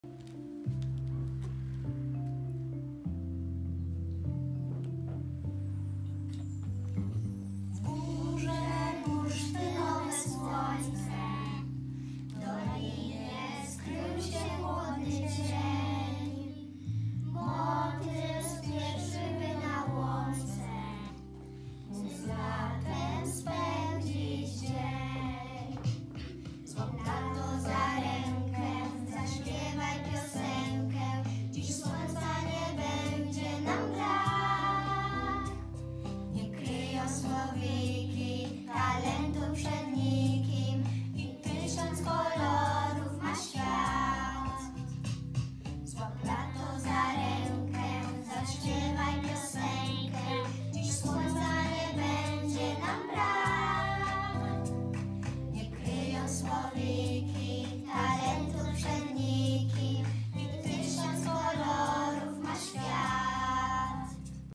Wakacyjna piosenka
Piosenkę zaśpiewały dzieci uczestniczące w półkolonii zorganizowanej przez opalenickie CKiB.